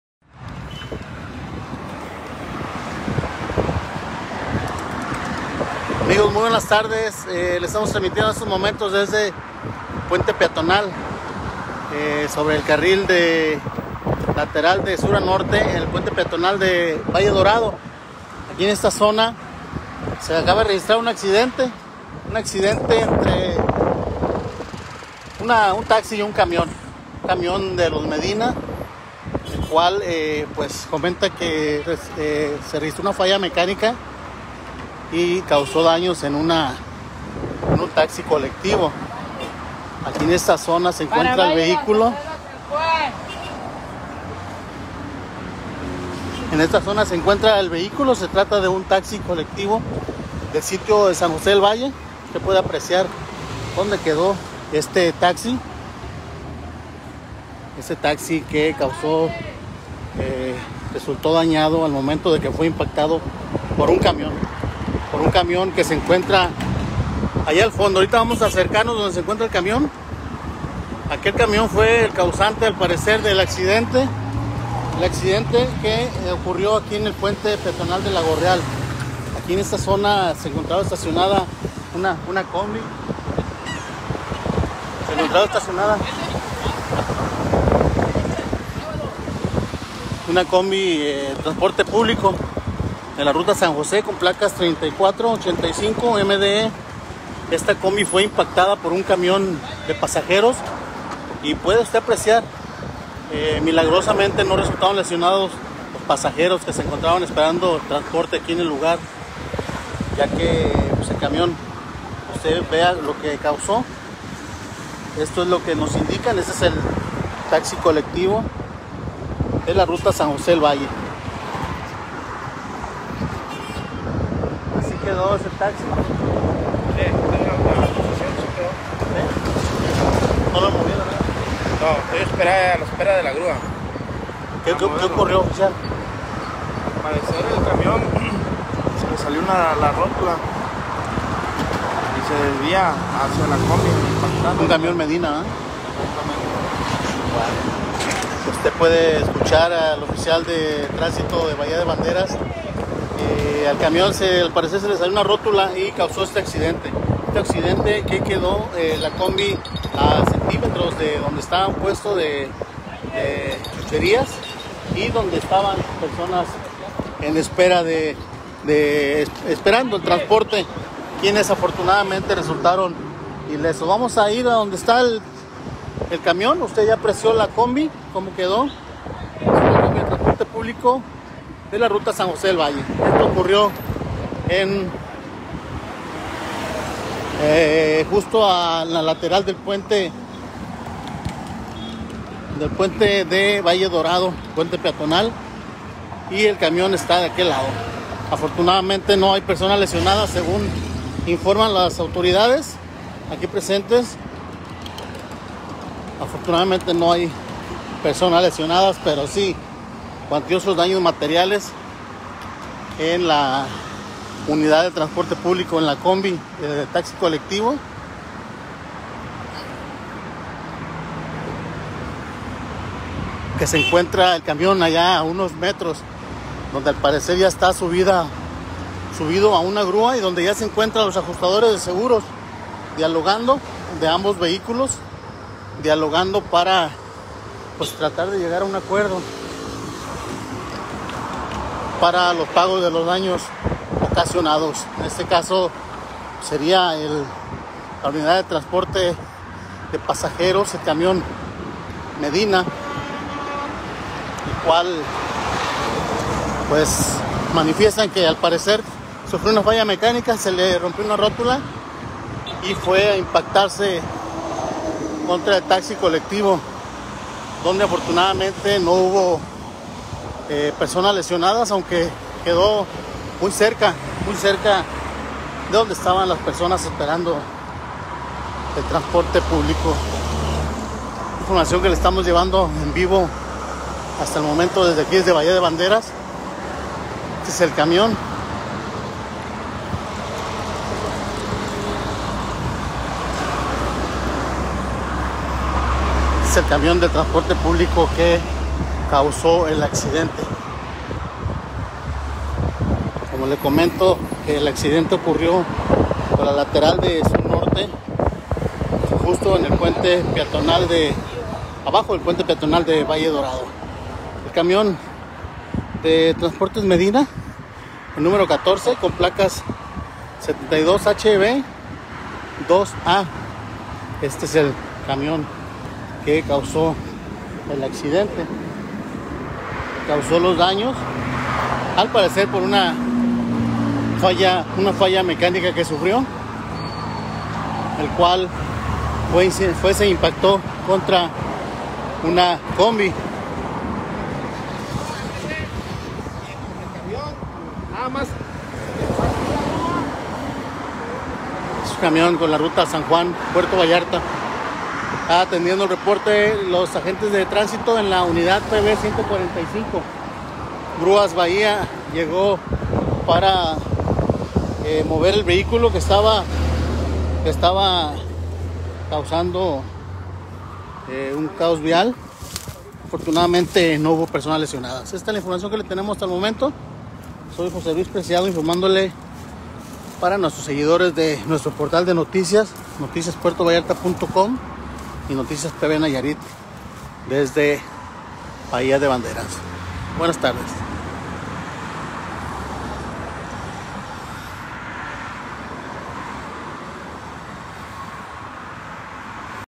📌Camión de pasajeros de autotransportes Medina sufrió una falla mecánica
Camión de pasajeros de autotransportes Medina sufrió una falla mecánica y causa de ello provocó un accidente que dejó daños a un taxi colectivo en la lateral sur a norte del Boulevard Riviera Nayarit, a la altura del puente peatonal de Valle Dorado, donde afortunadamente no hubo personas lesionadas, pero sí cuantiosos daños materiales, tomando nota los agentes de Tránsito Municipal.(José Luis Preciado/NoticiasPV.Com).